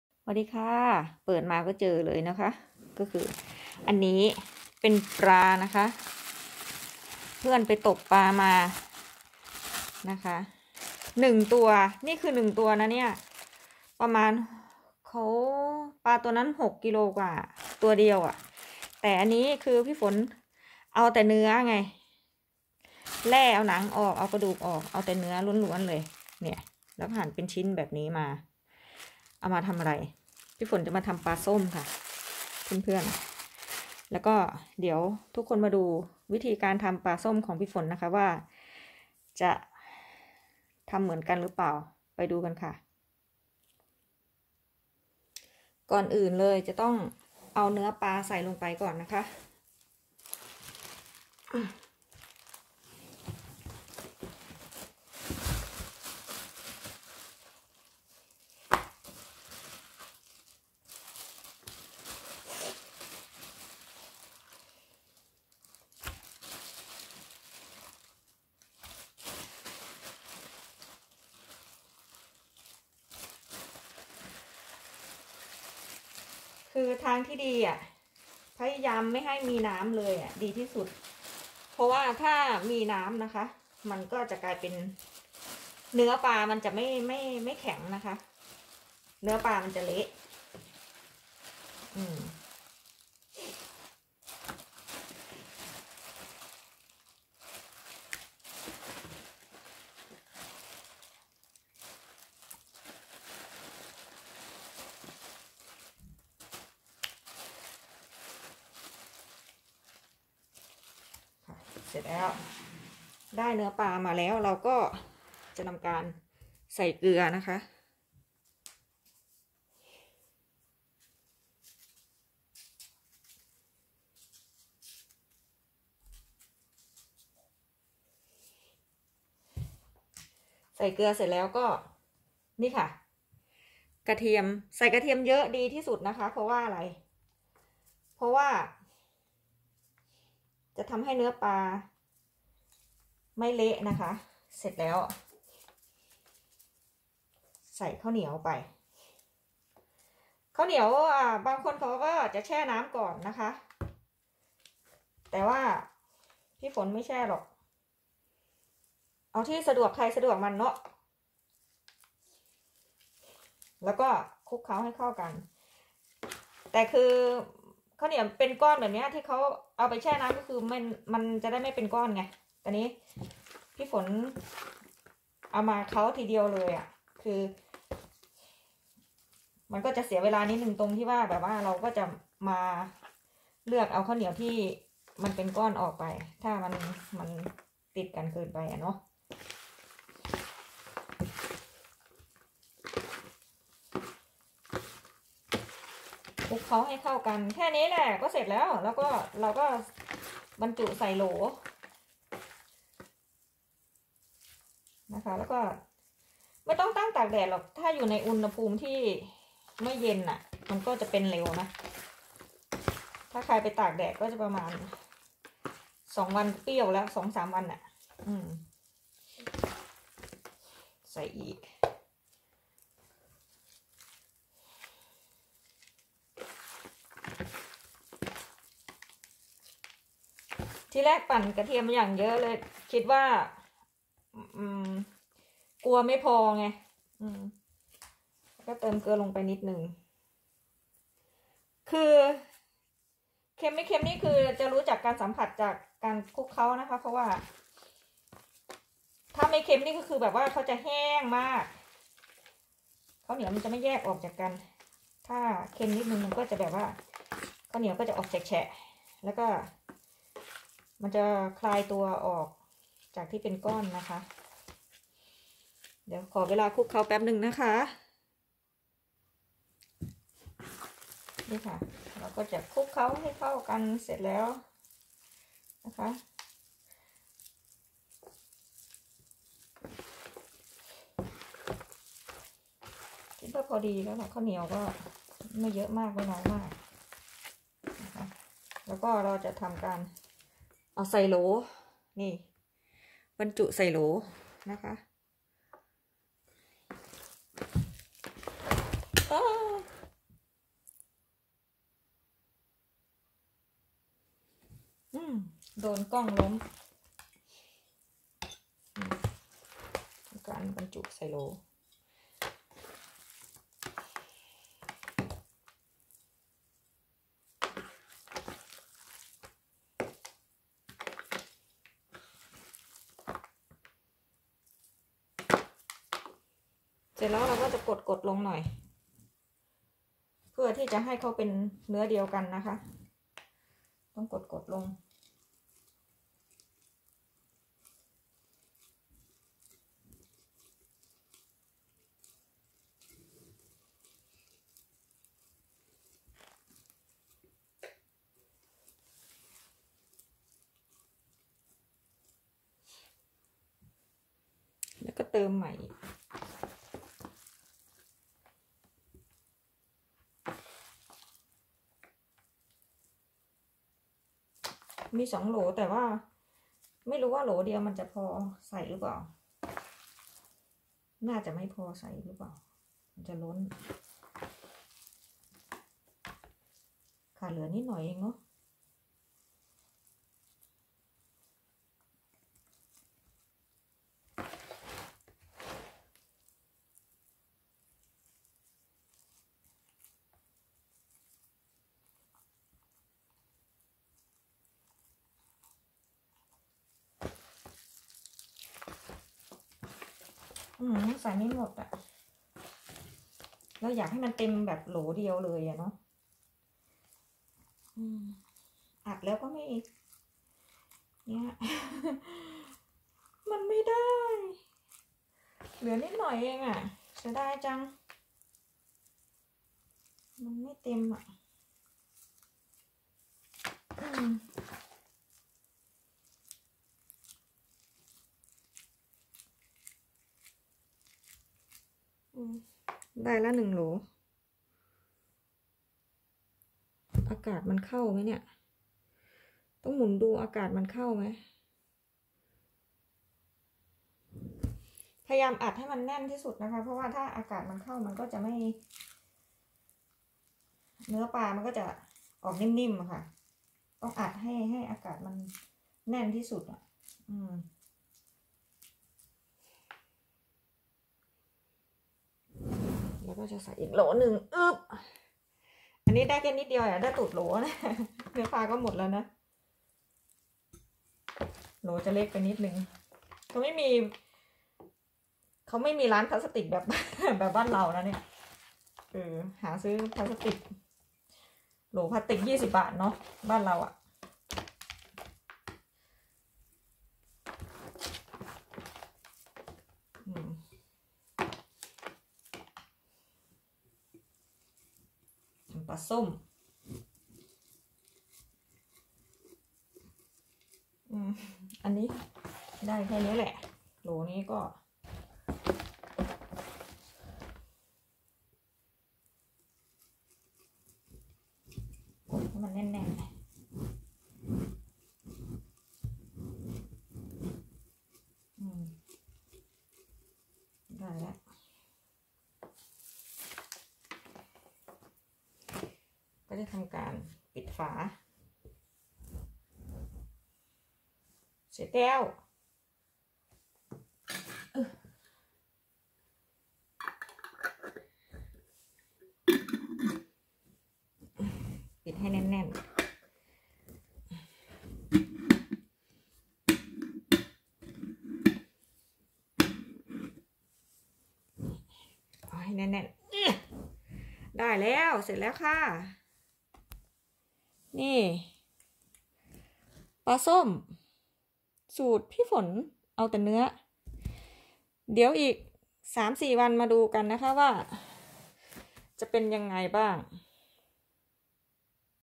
อยู่ต่างแดนก้อทำกินได้ปลาส้มจ้า
ทำกินเองประหยัดและอร่อย